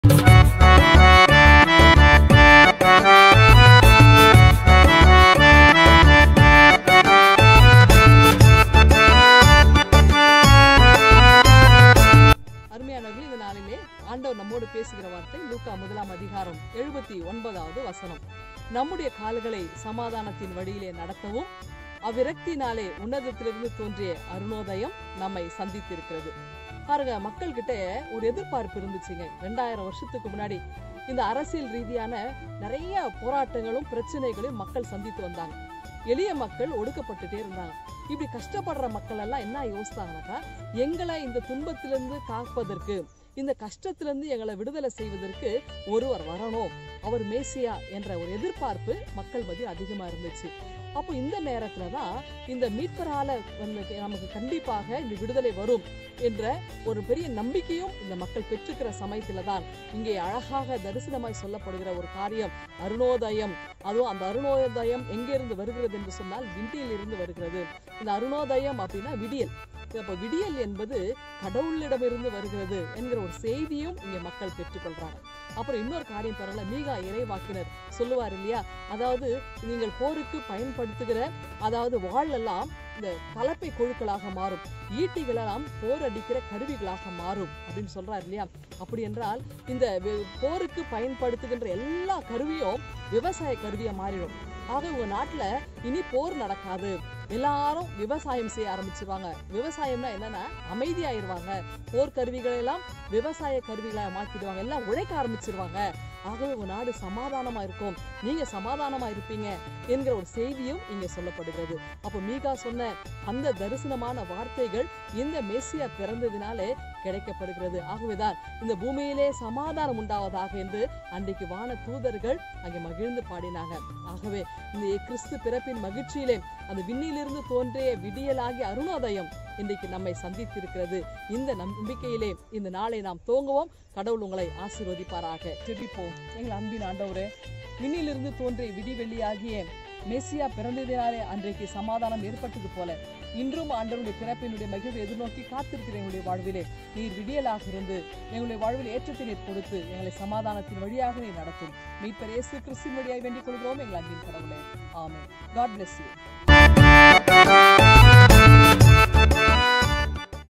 தினம் ஒரு சிந்தனை; 21.12.2020; Rev.S. CHRISTY MALARVIZHI, CSI PAMMAL PASTORATE.
லூக்கா 1.79
நம்முடைய கால்களைச் சமாதானத்தின் வழியிலே நடத்தவும், அவ்விரக்கத்தினாலே உன்னதத்திலிருந்து தோன்றிய அருணோதயம் நம்மைச் சந்தித்திருக்கிறது என்றான்.